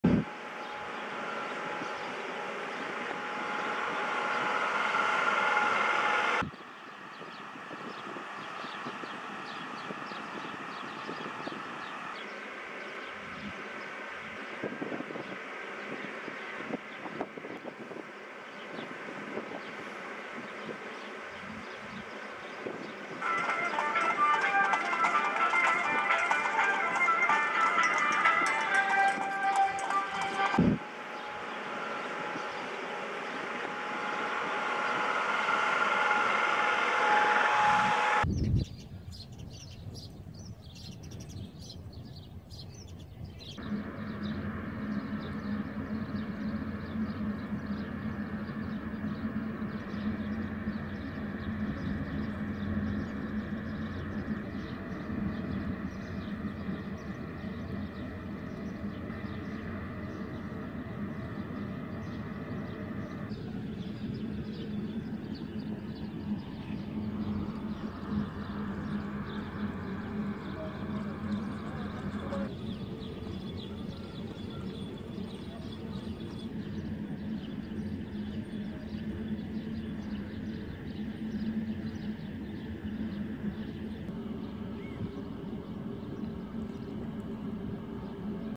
Samsun'daki pis kokunun nedeni bu gemi
Samsun'da iki gündür şehri etkisi altına alan kötü kokunun limanda bulunan 7 bin büyükbaş hayvan yüklü gemiden geldiği ortaya çıktı. 2 gün boyunca şehri gübre kokusu sardı.